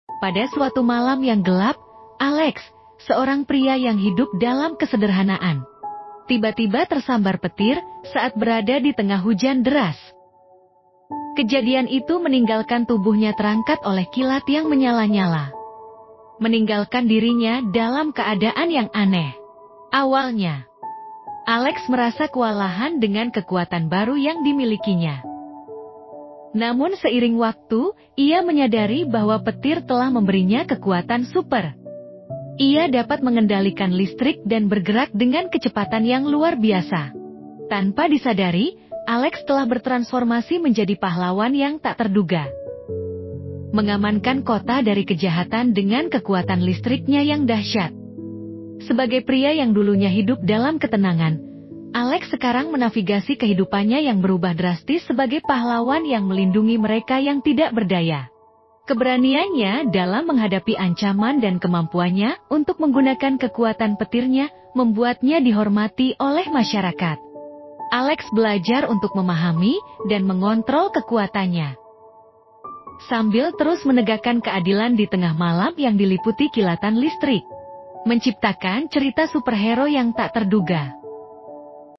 ALEX | CERITA PENDEK
HAI SEMUA.
PADA KESEMPATAN KALI INI ADA YANG BERBEDA NIH, JIKA SEBELUMNYA KONTENNYA KEBANYAKAN TENTANG MEWARNAI, MAKA KALI INI SI ADMIN PENGEN COBA BUAT CERITA-CERITA SINGKAT.

MOHON MAAF ATAS SEGALA KEKURANGAN.